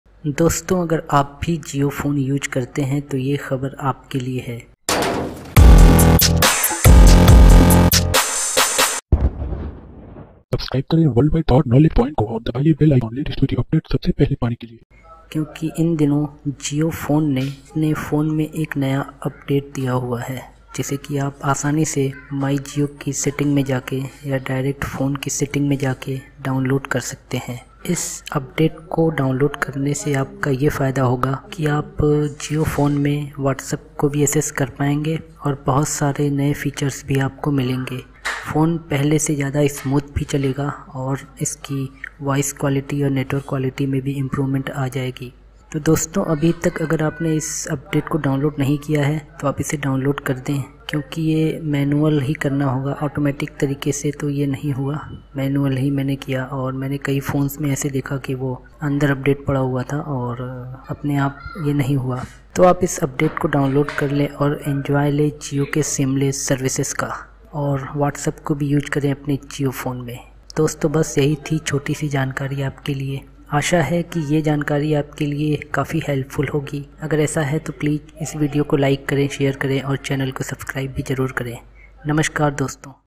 Jio Phone Update \ Jio phone me aaya ek new update | jio phone Whatsapp
Jio Phone Update, Jio phone me aaya ek new update | jio phone Whatsapp. Doston reliance Jio India ne apne 500 wale Jio phone me ek naya update diya hai jise aapko manual hi Jio phone ki setting me jakar ke download karna hoga. Is update ko download karne ka benefit ye hoga ki aapka jio phone pahle se kahi achha perform karega, jaise ki acchi speed aur acchi voice quality bahot kuch aur sabse interesting baad ye hai ki is update ko download karne ke baad aap apne Jio phone me Whatsapp ko bhi use kar payenge aur wo bhi kafi acchi speed ke sath to doston abhi tak agar aapne Jio phone ke is update ko download nahi kiya hai to please aap Jio phone ki setting me jakar is new Jio update ko download kar le.